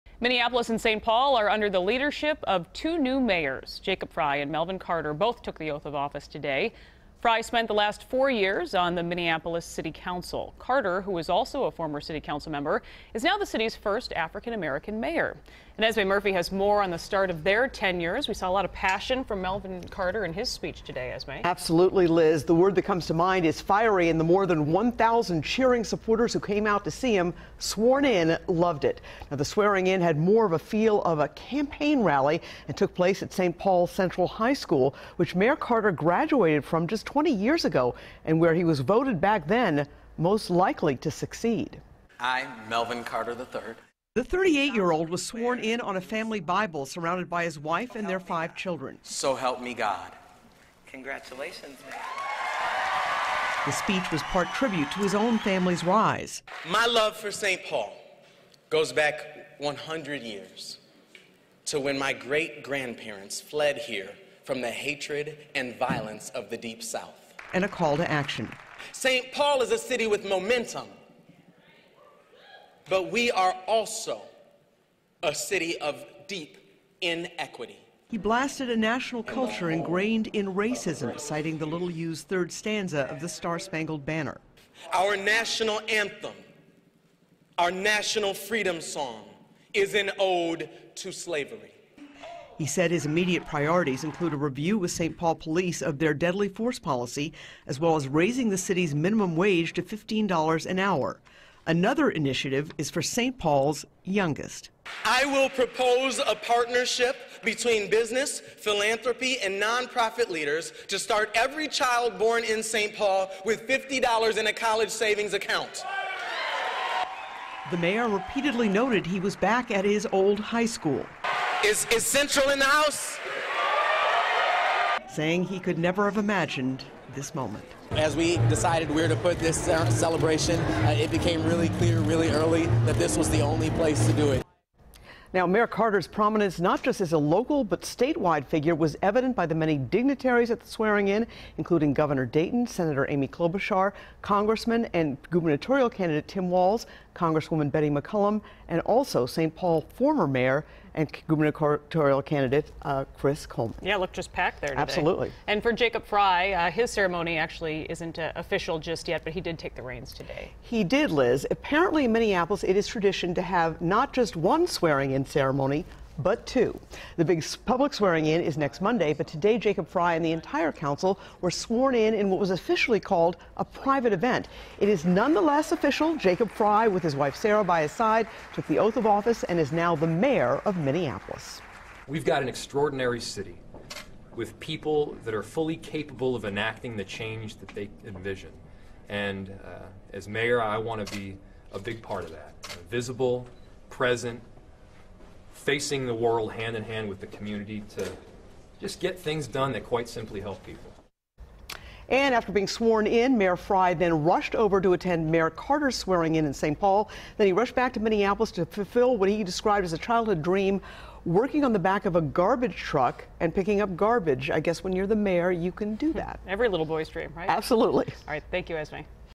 In Inauguration Speech, Melvin Carter Addresses St. Paul’s ‘Deep’ Inequities
Carter was sworn in as mayor at St. Paul’s Central High School, where he graduated from 20 years ago, Esme Murphy reports (3:39). WCCO 4 News At 5 – January 2, 2018